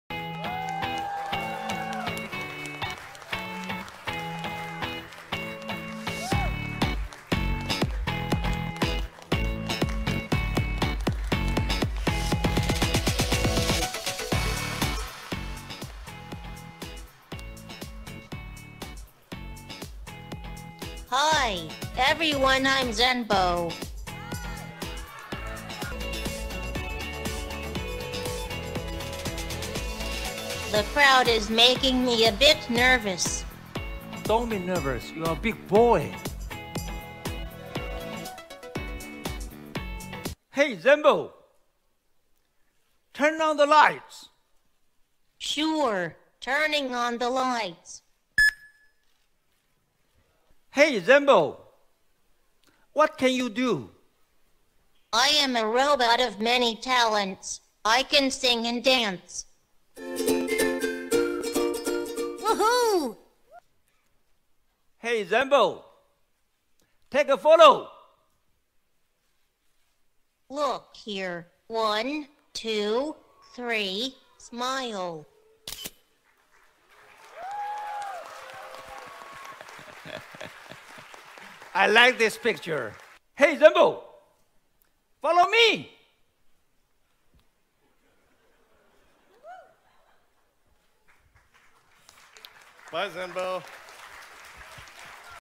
Sekitar Pengenalan Asus Zenbo - Robot Peribadi Dari Asus
Asus hari ini telah memperkenalkan robot peribadi mereka kepada pengguna umum yang mana boleh melakukan beberapa perkara, termasuk mendengar arahan suara, mengawal peralatan elektronik, merakamkan gambar dan menghubungi keluarga ketika kecemasan.